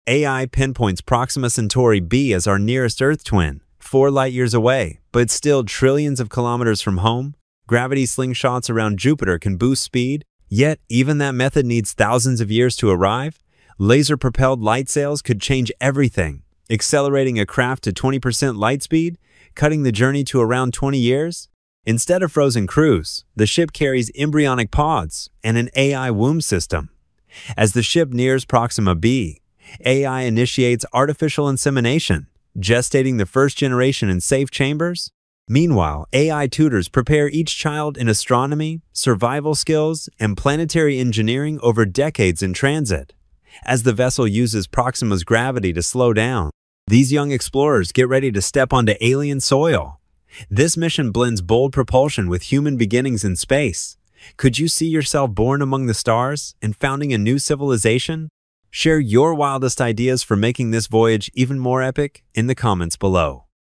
Travelling To The Closest Earth Like Planet?
AI answers the question: Travelling To The Closest Earth Like Planet?

What is your take? Write a comment!